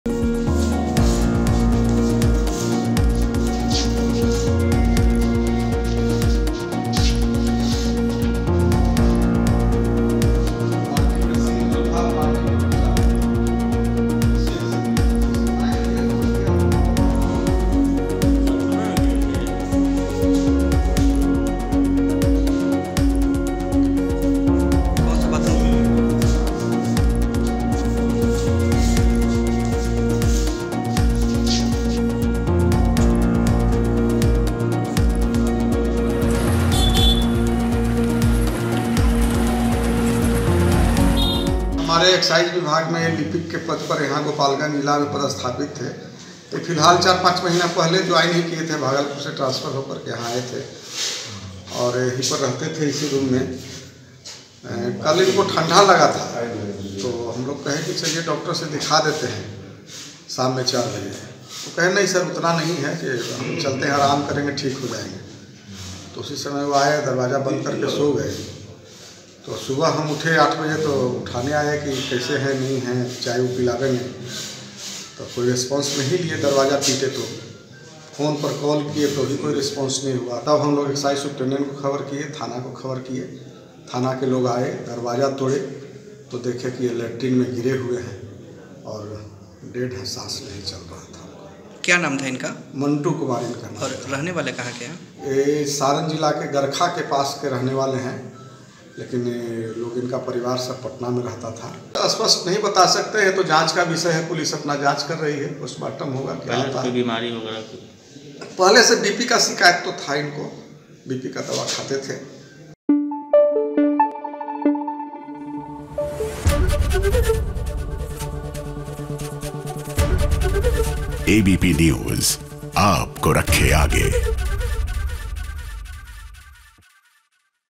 Gopalganj में उत्पाद विभाग के क्लर्क का बाथरूम में मिला शव, पुलिस ने पोस्टमार्टम के लिए भेजा
बिहार के  गोपालगंज में एक उत्पाद विभाग के क्लर्क की संदिग्ध हालात में मौत हो गई है. क्लर्क का रात को सोए थे लेकिन मकान के बाथरूम में सुबह शव मिलने से सनसनी फैल गई है. पुलिस ने शव को कब्जे में लेकर पोस्टमार्टम के लिए भेज दिया है. क्लर्क के साथी ने बताया वो बीपी की दवाई खाते थे.